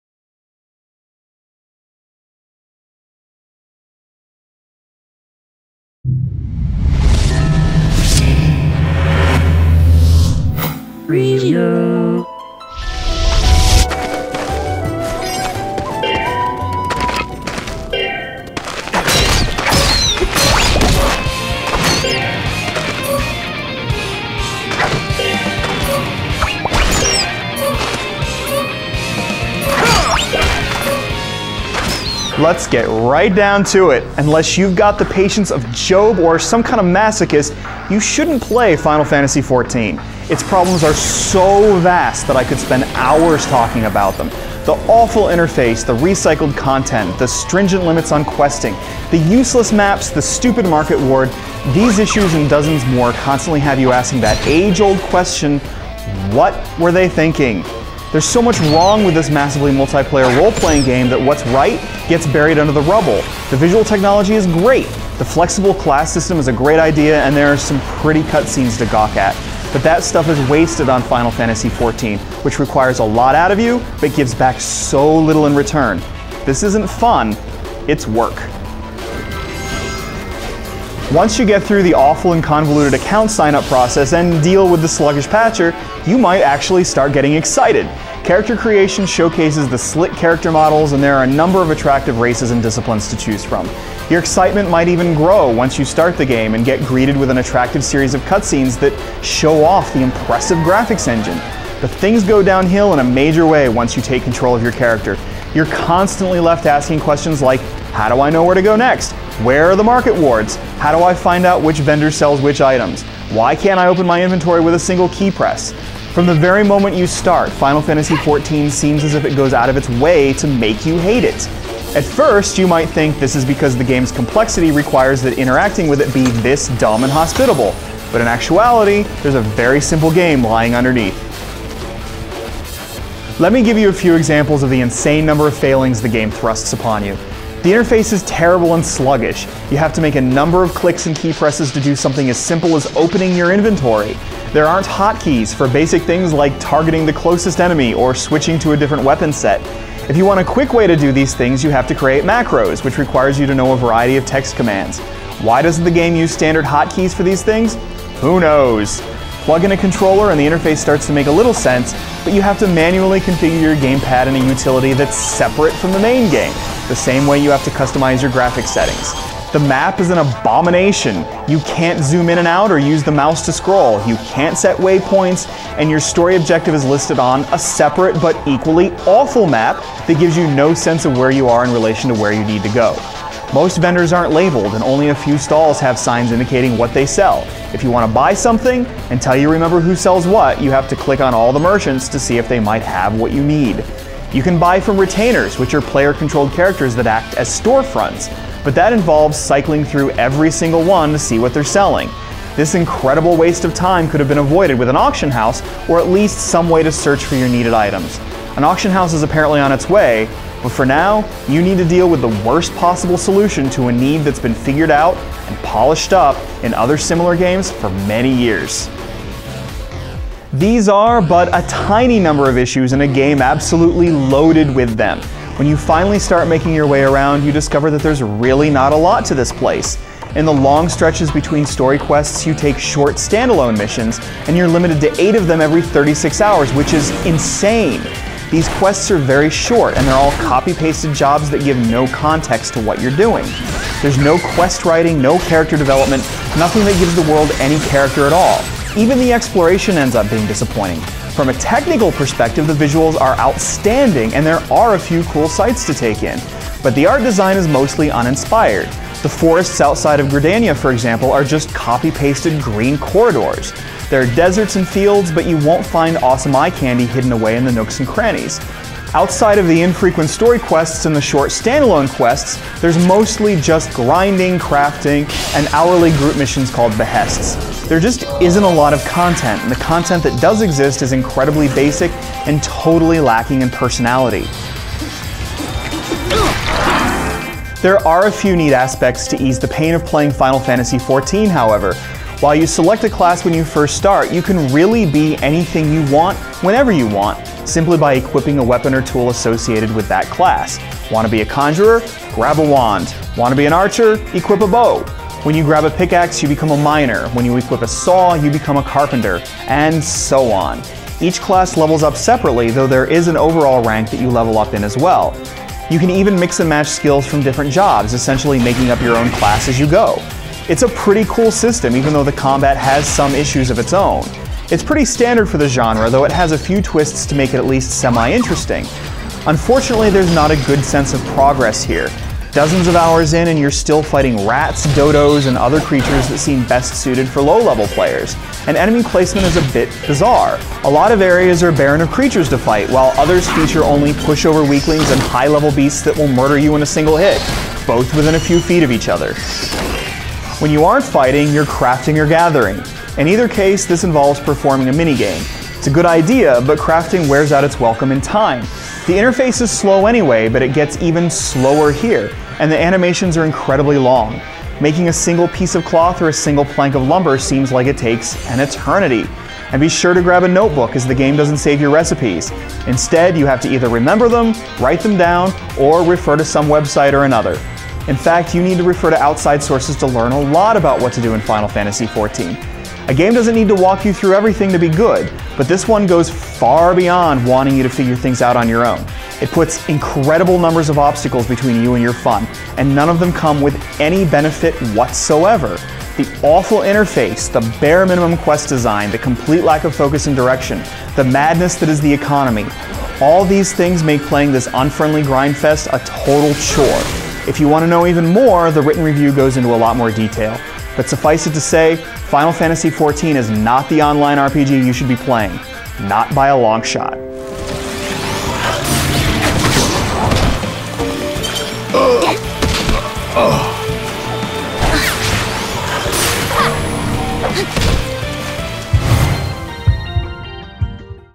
GameSpot Reviews - Final Fantasy XIV Online Video Review
Square Enix's laborious online role-playing game is a step backward for the genre.